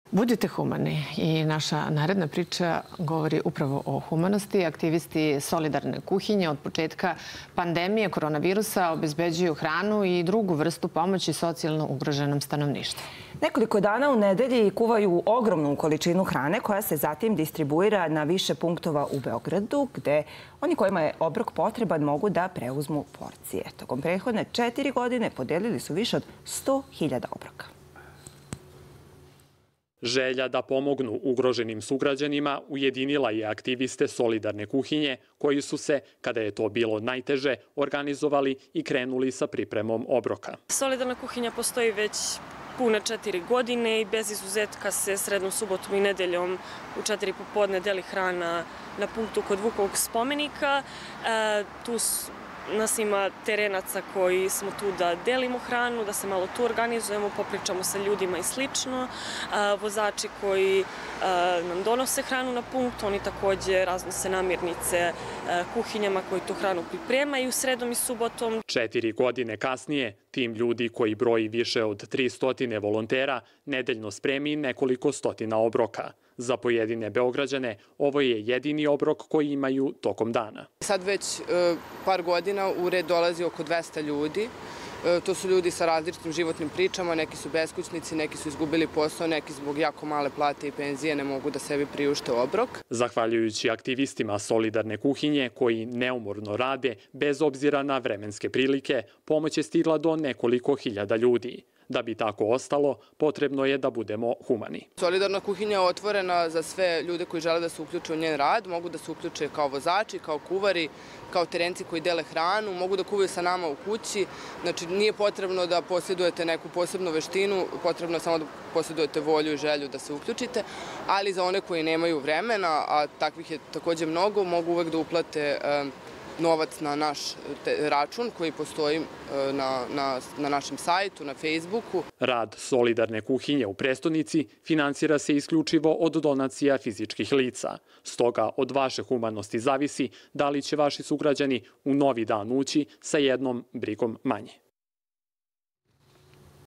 Solidarna kuhinja
Aktivisti  "Solidarne kuhinje" od početka pandemije koronavirusa obezbeđuju hranu i drugu vrstu pomoći socijalno ugroženom stanovništvu.  Nekoliko dana u nedelji kuvaju ogromnu količinu hrane, koja se zatim distribuira na više punktova u Beogradu, gde oni kojima je obrok potreban mogu da preuzmu porcije. Tokom prethodne četiri godine podelili su više od 100.000 obroka.